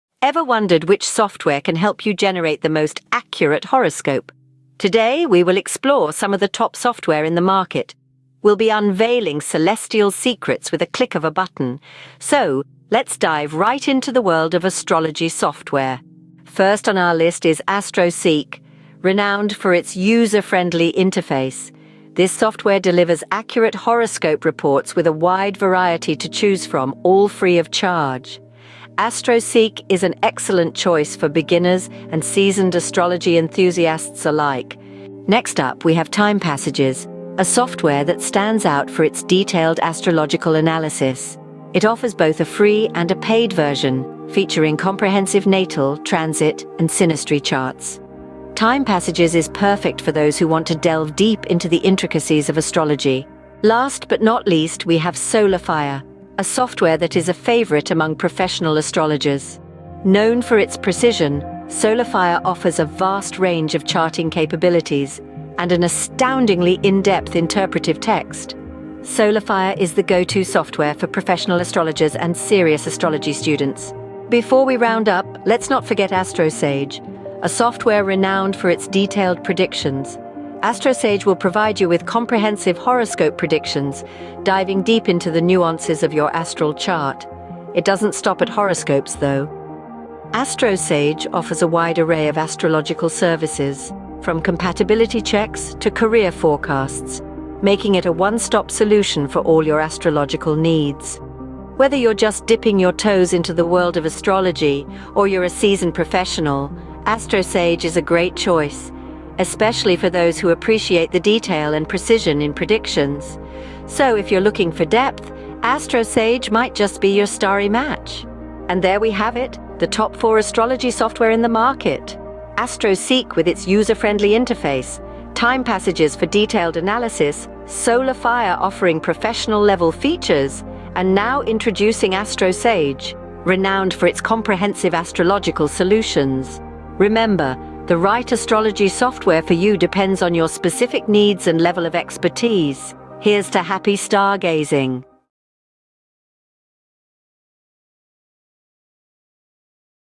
Top Astrology Software
OUTLINE:
00:00:00
Introduction

00:00:16
AstroSeek

00:00:35
TimePassages

00:00:53
Solar Fire

00:01:14
AstroSage Unveiled

00:01:58
Conclusion